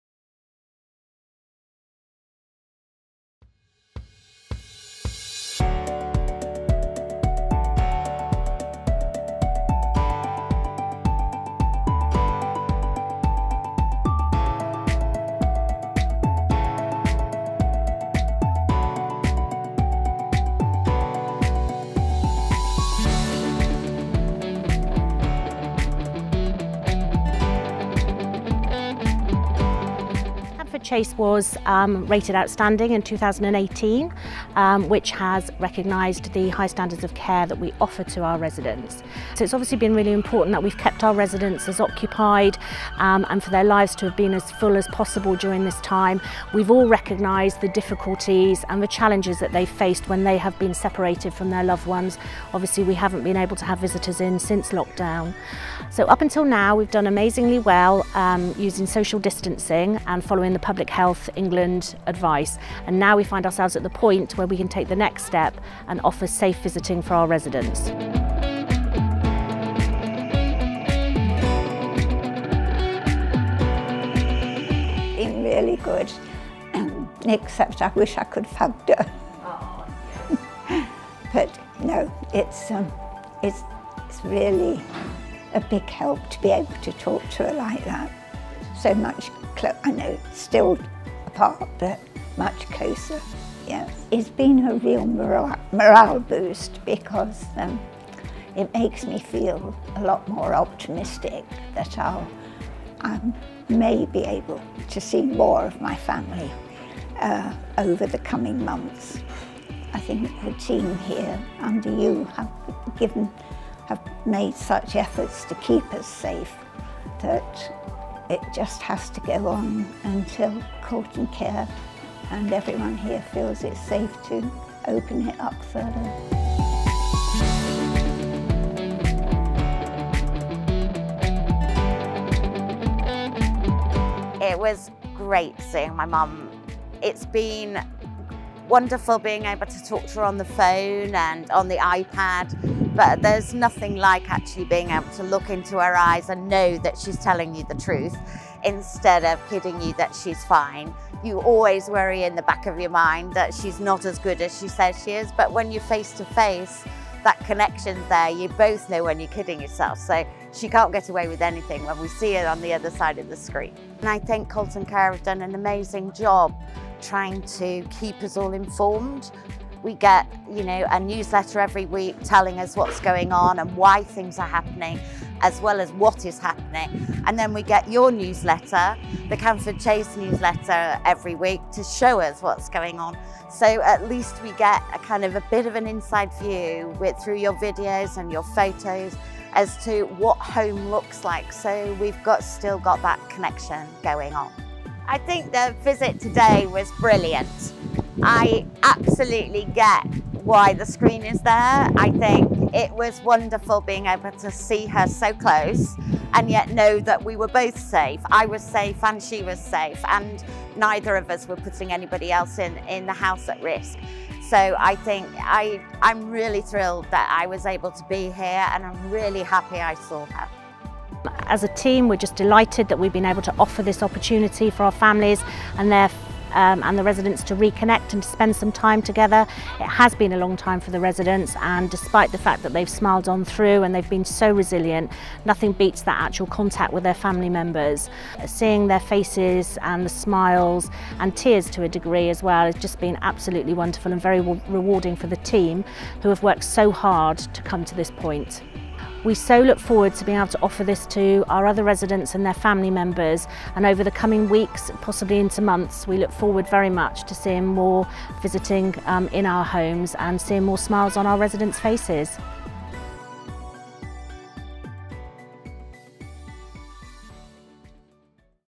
Safe reunions at Colten Care
Residents at all Colten Care homes have been reunited with their loved ones thanks to a new Covid-safe visiting approach.